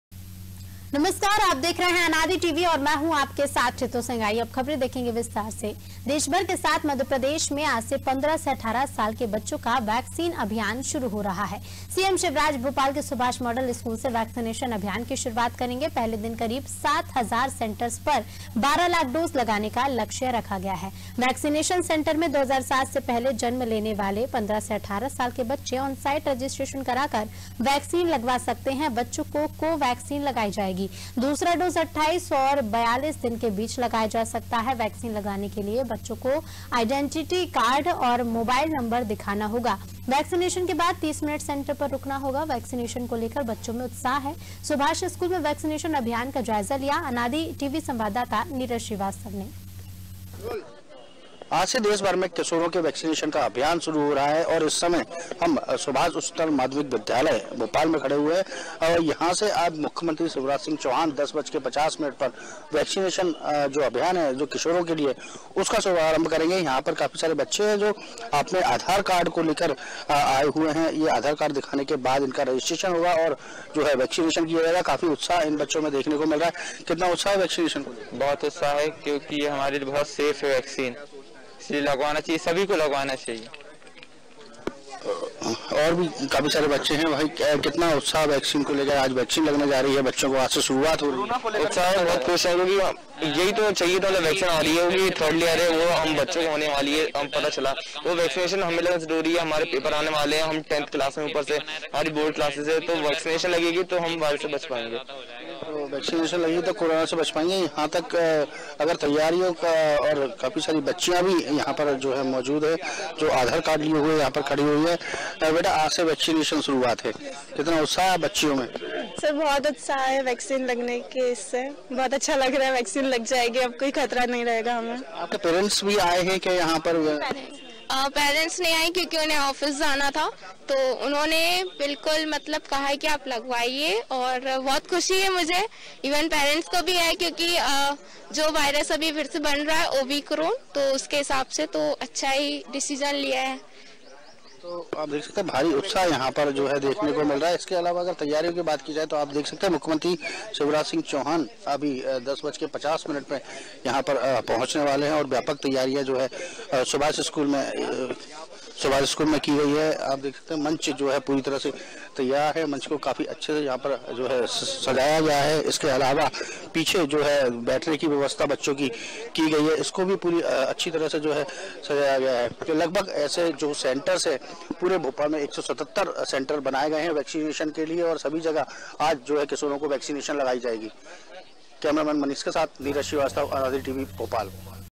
BHOPAL : बच्चों का वैक्सीनेशन आज से | MP News | Hindi news | Latest news | Anaadi TV
देशभर के साथ एमपी में आज से 15 से 18 साल के बच्चों का वैक्सीन अभियान शुरू हो रहा है... सीएम शिवराज  भोपाल के सुभाष मॉडल स्कूल से वैक्सीनेशन अभियान की शुरुआत करेंगे... पहले दिन करीब 7 हजार सेंटर पर 12 लाख डोज लगाने का लक्ष्य रखा गया है... वैक्सीनेशन सेंटर में 2007 से पहले जन्म लेने वाले 15 से 18 साल के बच्चे ऑनसाइट रजिस्ट्रेशन कराकर वैक्सीन लगवा सकते हैं... बच्चों को कोवैक्सीन लगेगी... दूसरा डोज 28 से 42 दिन के बीच लगवाया जा सकता है... वैक्सीन लगाने के लिए बच्चों को आइडेंटिटी कार्ड और मोबाइल नंबर दिखाना होगा... वैक्सीनेशन के बाद 30 मिनट सेंटर पर ही रुकना होगा...वैक्सीनेशन को लेकर बच्चों में उत्साह है... सुभाष स्कूल में वैक्सीनेशन अभियान का जायजा लिया अनादि रिपोर्टर नीरज श्रीवास्तव ने...